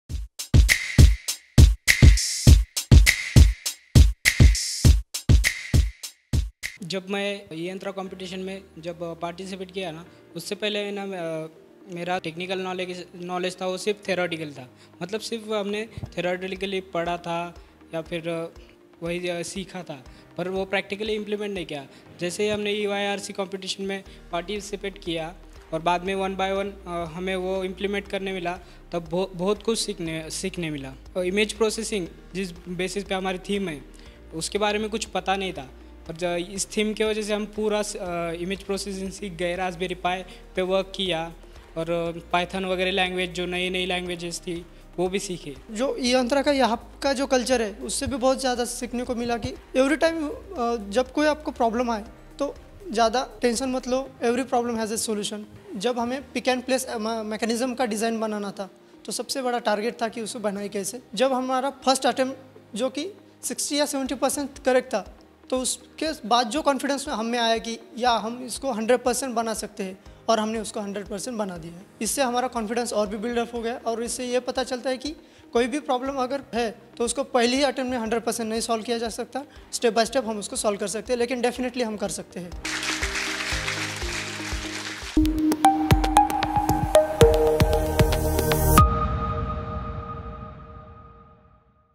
eYRC 2018: Ant Bot Theme Interview (Government College of Engineering & Research, Avasari Khurd)
Student team from Government College of Engineering & Research, Avasari Khurd qualified for the National Finals at IIT Bombay.